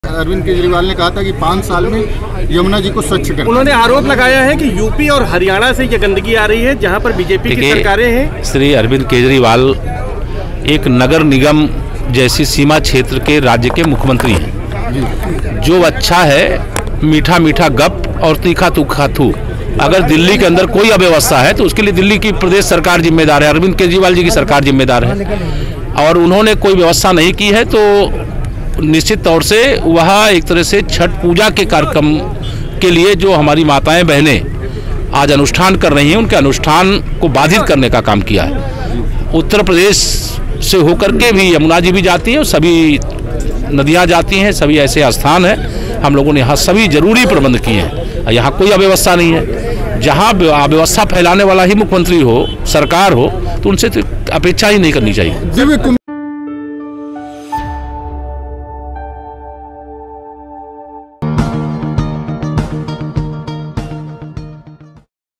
Yamuna Pollution in Delhi: यमुना में प्रदूषण के लिए Keshav Maurya का Arvind Kejriwal पर निशाना | NBT
दिल्ली में यमुना नदी के प्रदूषण को लेकर राजनीतिक बयानबाजी तेज हो गई है। दिल्ली के सीएम अरविंद केजरीवाल ने यमुना में प्रदूषण के लिए यूपी और हरियाणा की बीजेपी सरकारों को जिम्मेदार ठहराया था। इस पर पलटवार करते हुए यूपी के डेप्युटी सीएम केशव प्रसाद मौर्य ने कहा कि अरविंद केजरीवाल की आदत है- मीठा मीठा गप और कड़वा कड़वा थू। अगर दिल्ली के अंदर कोई अव्यवस्था है तो उसके लिए दिल्ली की अरविंद केजरीवाल सरकार ही जिम्मेदार है।